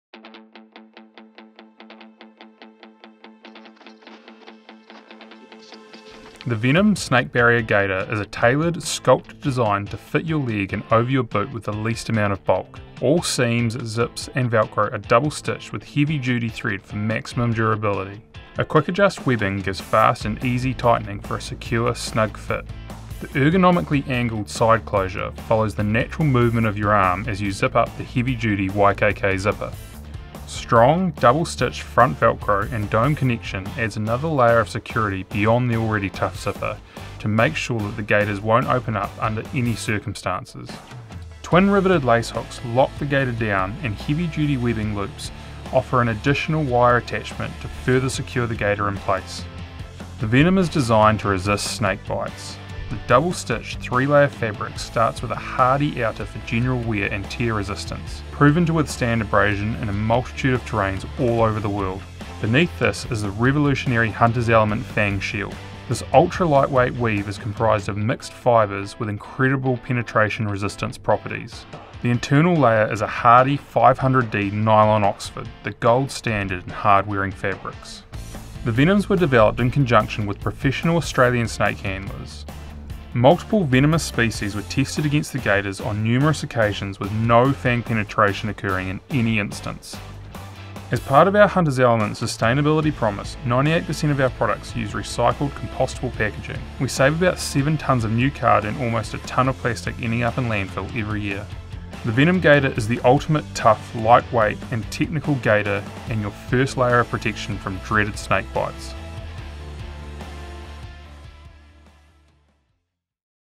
Venom Gaiter V2
The world’s best snake gaiters have been upgraded and refined allowing you to attack the Aussie bush in snake season. The Venom Gaiter V2 takes the fear out of missions in the heart of snake country thanks to the incredible penetration resistance properties of the Fang Shield™ internal membrane.

Features:
Improved Outer Fabric
This innovative material is lighter with increased noise suppression and resistance to tears and annoying Aussie burrs/seeds.

New Calf Panel
Contouring the gaiter with the shape of your leg to reduce friction and increase comfort.

Extra Ankle Dome
Two military-grade ankle domes complement the YKK® zip and Velcro® triple closure to ensure the gaiters remain secure all day.

Fang Shield Membrane
Our internal membrane was developed and tested on snake fangs from a variety of Australian species including Red Belly Blacks and King Browns.

Increased Lace Hook Durability
Widened tabs increase the durability and longevity of the twin riveted lace hooks.

Quick Adjust Webbing
Makes it quick and easy to adjust the gaiter for the perfect fit.

Heavy Duty Webbing Loops
Are compatible with our Gaiter Wires to stop the gaiters from riding up.